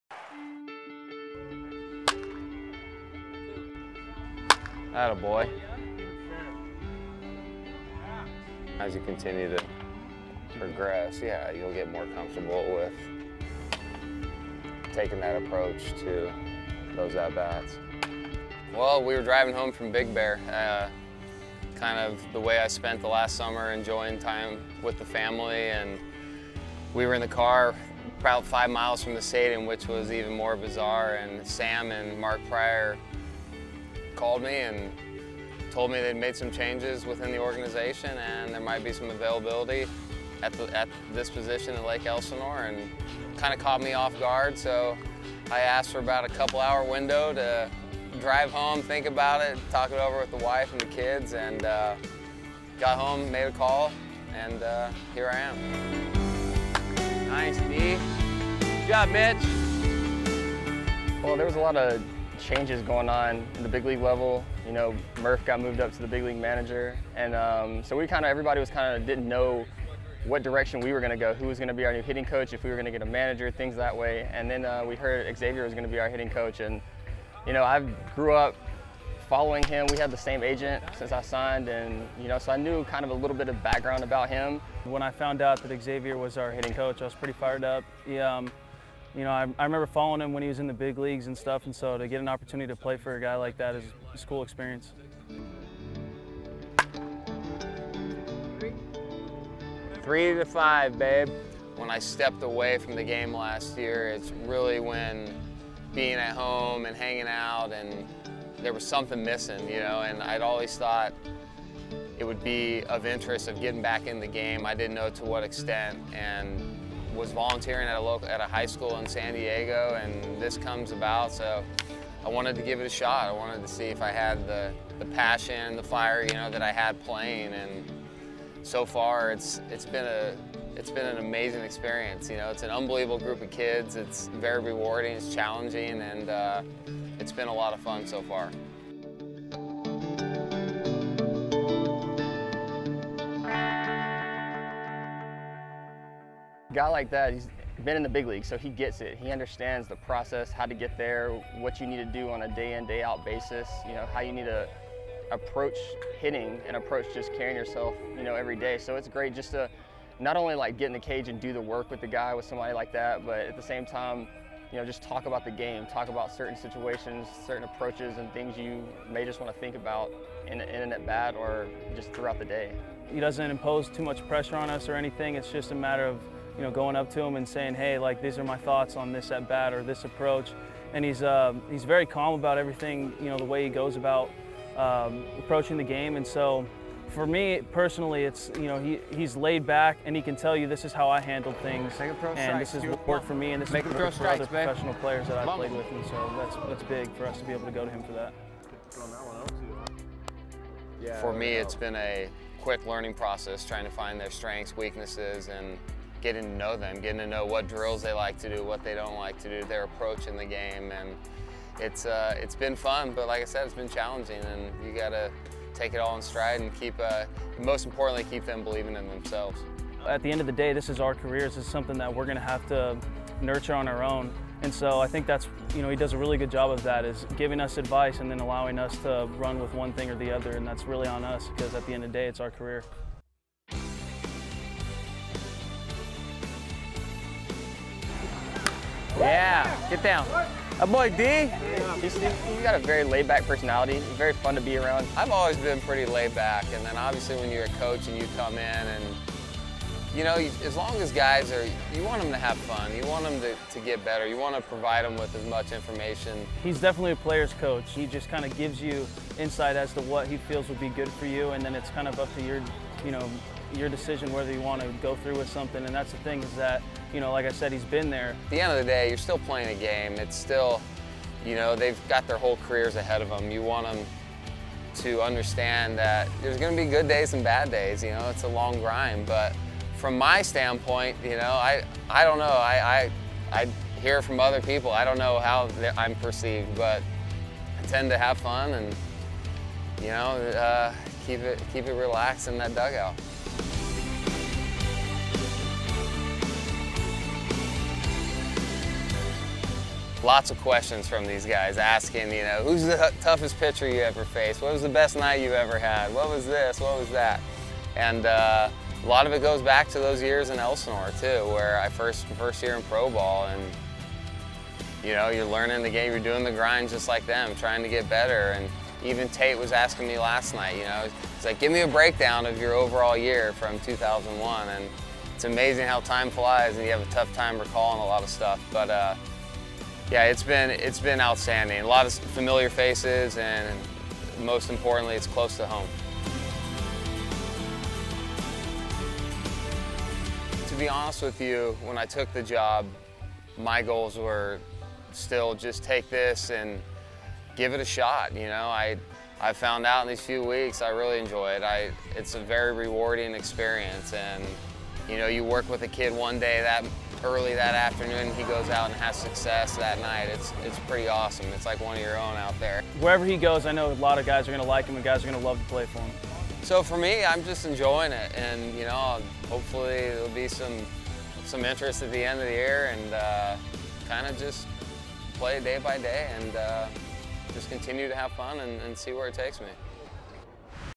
Xavier Nady tells his story
Xavier Nady, who played parts of his 12-year MLB career with the Padres, became the Storm's hitting coach in June of 2015, and shared with Padres POV what it was like making that transition from a player.